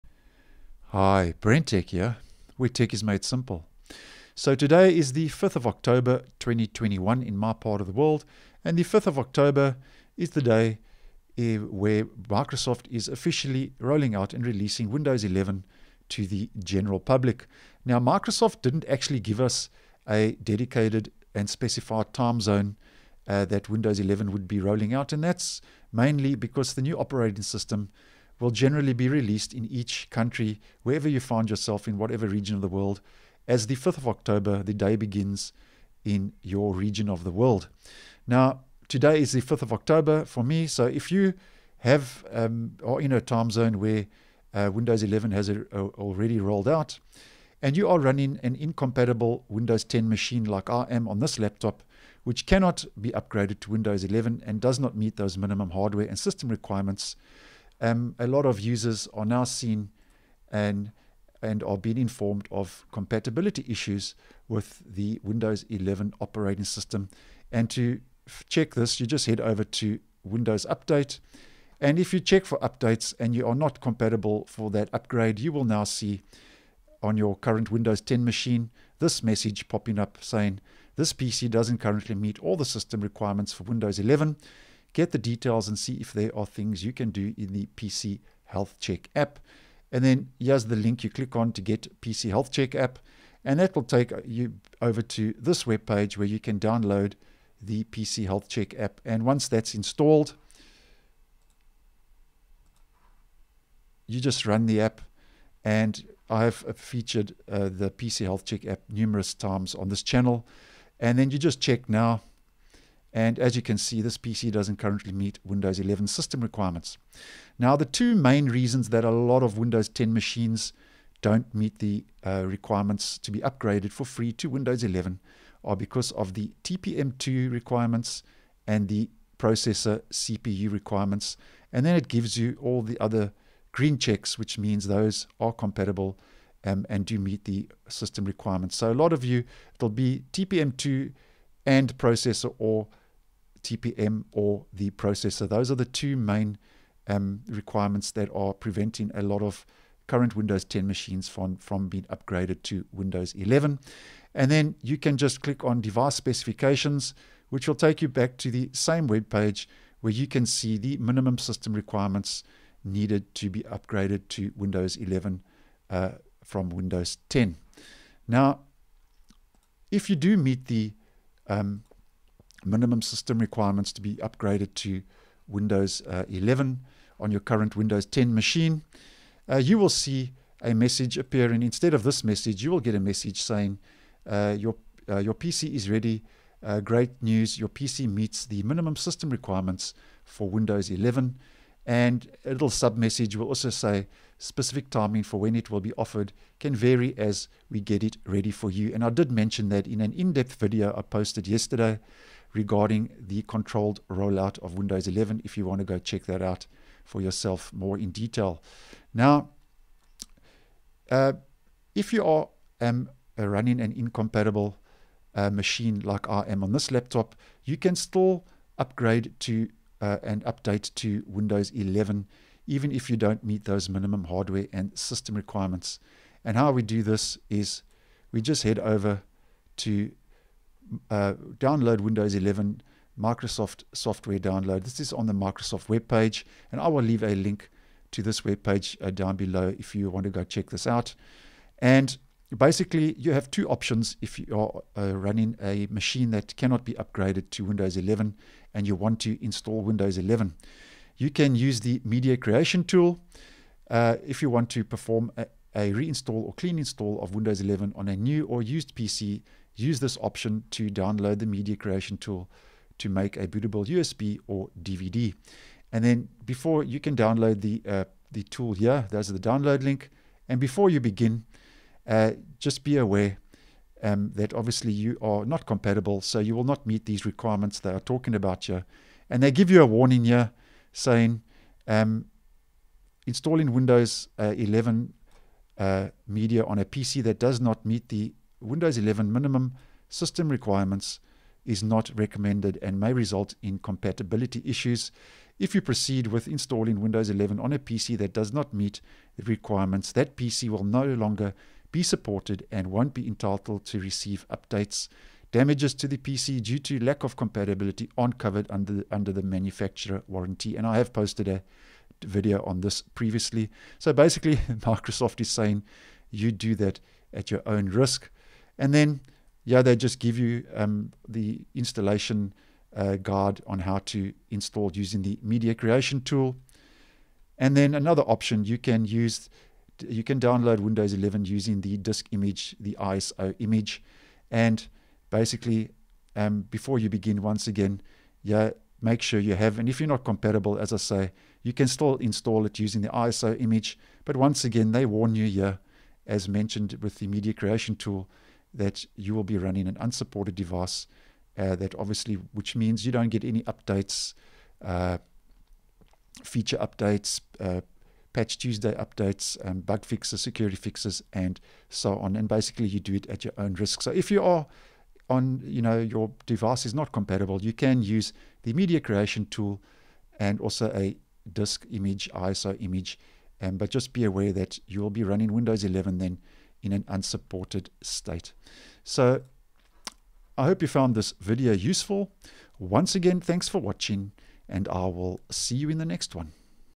Windows 11 is now rolling out with a compatibility error message now displaying on incompatible PCs
If you are running unsupported hardware you could now be informed of compatibility issues with the Windows 11 operating system. If you have incompatible hardware you can still install Windows 11 at your own risk using the Media Creation Tool or an ISO image file: